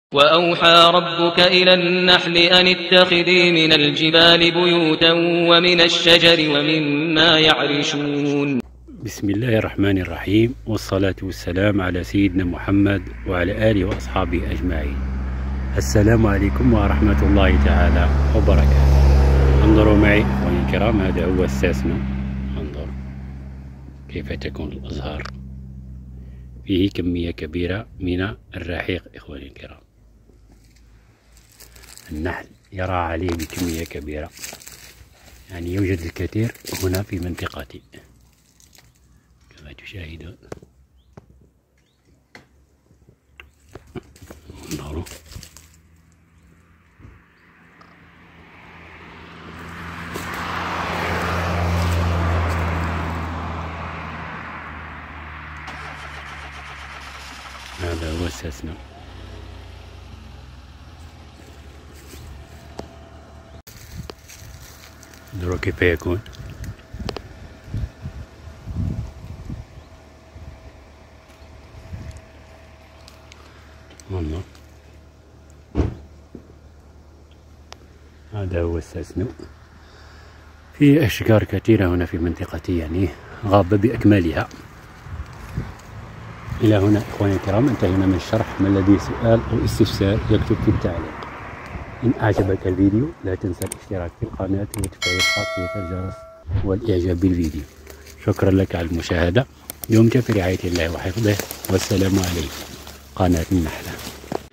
شجرة الساسنو فيها كمية كبيرة من الرحيق والنحل يقبل عليها بكثرة
هذه الشجرة تزهر في فصل الخريف ويجلب منها النحل كمية كبيرة من حبوب اللقاح والرحيق وينتج منها العسل المر وتوجد بكثرة  في المناطق الساحلية .
وتنتج فاكهة لونها حمراء وناكلها .
أرجوا منكم ان تدعموني بمشاركة الفيديوهات معأصدقائكم لتعم الفائدة وأنال عدد كبير من المتتبعين ويعلم كل الناس تربية النحل الصحيحة.